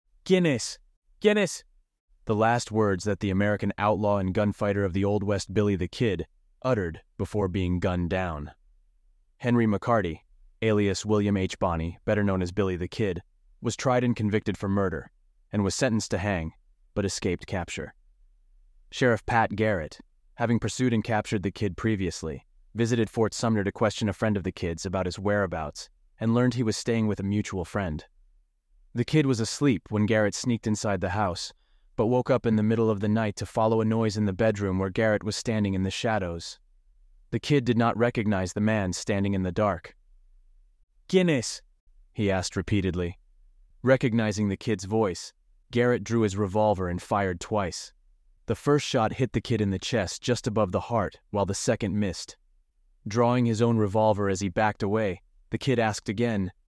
The Last Words of Billy the Kid (on this day 14 July 1881)
"¿Quién es?" "¿Quién es?" The last words that the American outlaw and gunfighter of the Old West Billy the Kid uttered before being gunned down. Henry McCarty, alias William H. Bonney, better known as Billy the Kid, was tried and convicted for murder and was sentenced to hang but escaped capture. Sheriff Pat Garrett, having pursued and captured the Kid previously, visited Fort Sumner to question a friend of the Kid's about his whereabouts and learned he was staying with a mutual friend. The Kid was asleep when Garret sneaked inside the house, but woke up in the middle of the night to follow a noise in the bedroom where Garrett was standing in the shadows. The Kid did not recognize the man standing in the dark. "Who is it?" he asked repeatedly. Recognizing the Kid's voice, Garrett drew his revolver and fired twice. The first shot hit the Kid in the chest just above the heart, while the second missed. Drawing his own revolver as he backed away, the Kid asked again: